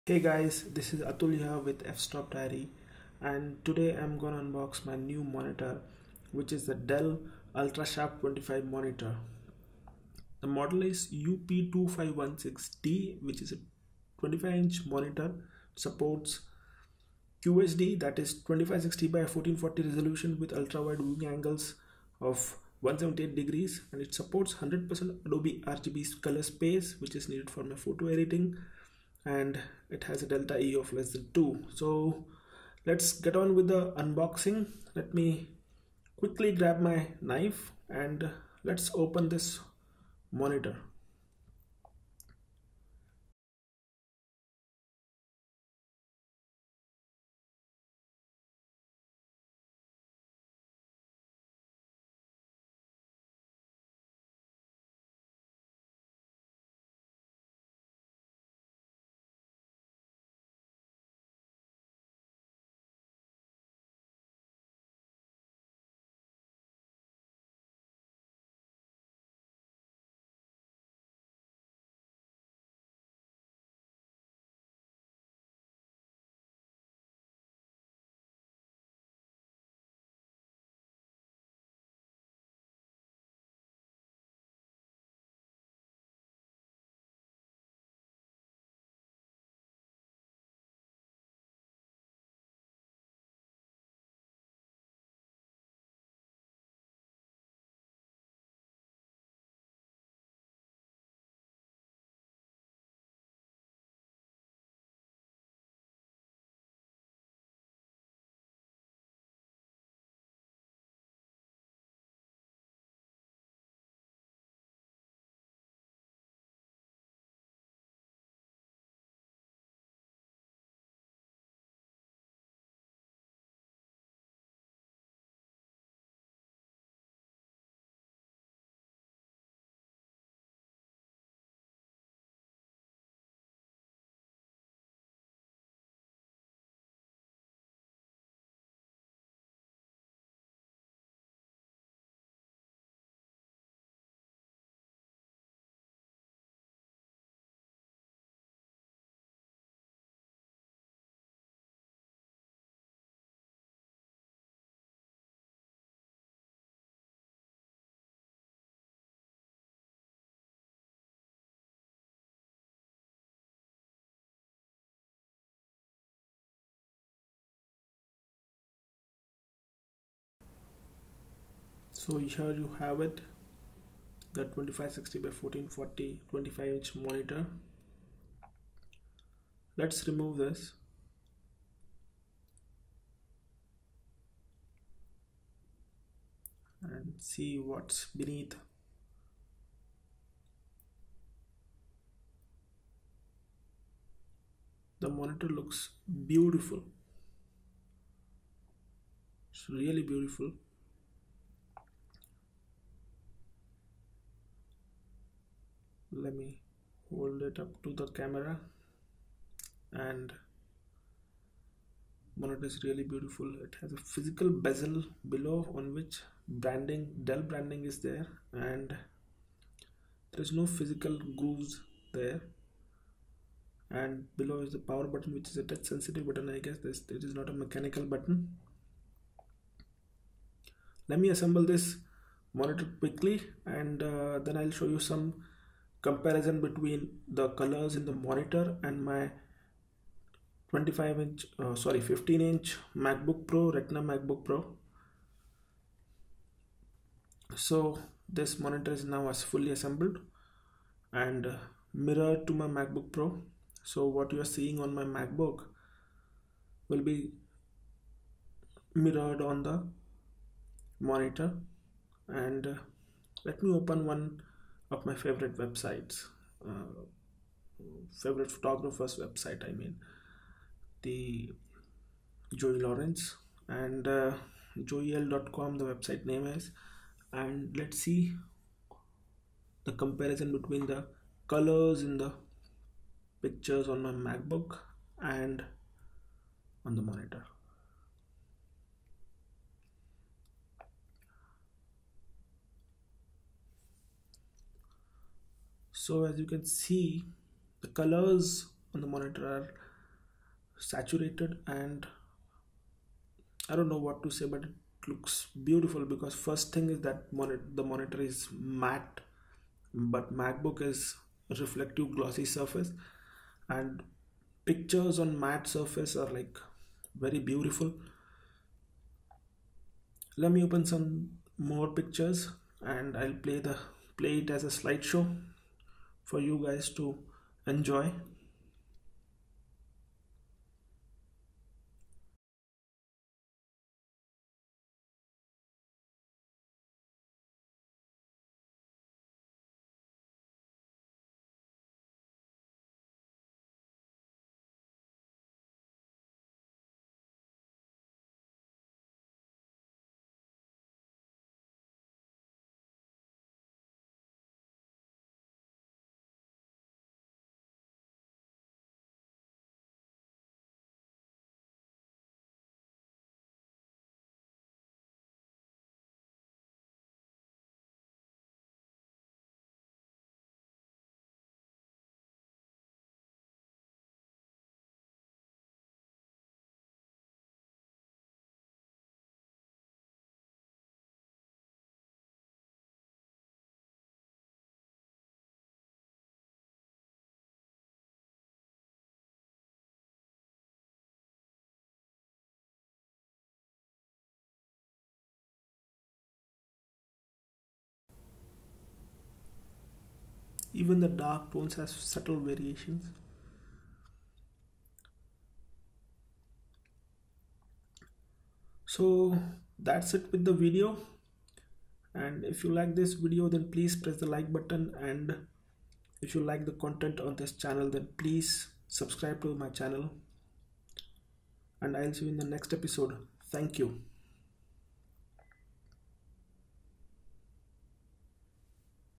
Best Budget Monitor for Photo Editing - Dell Ultrasharp UP2516D 25" QHD Monitor
Dell Ultrasharp UP 2516 D QHD Monitor is the budget Friendly Monitor which supports 100% Adobe RGB Color Space.